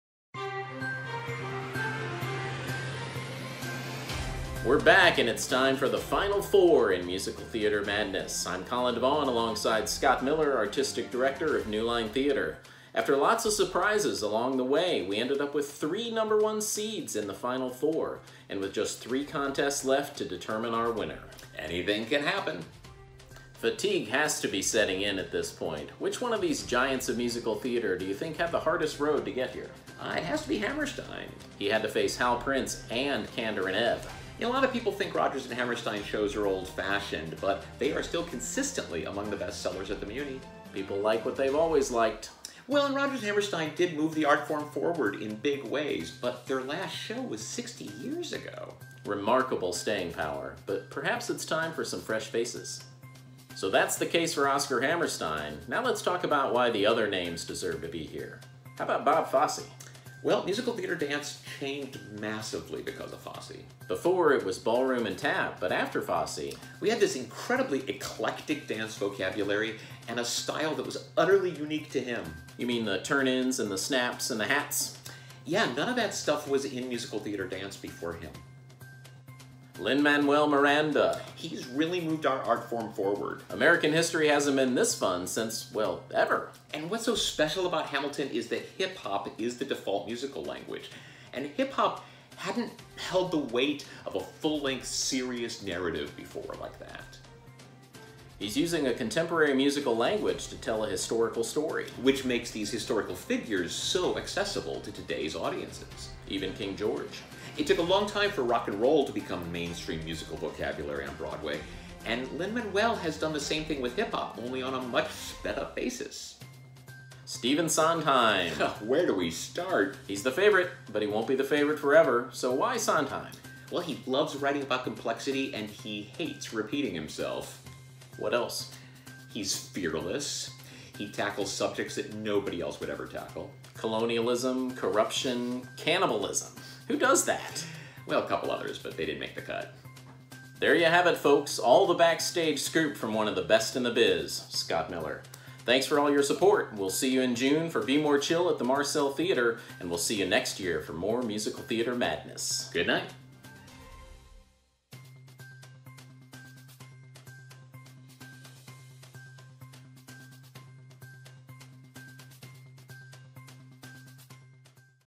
MusicalTheatreMadness FINAL FOUR
Colin and Scott review the FINAL FOUR!

Vote for your favorites on New Line Theatre's FB page!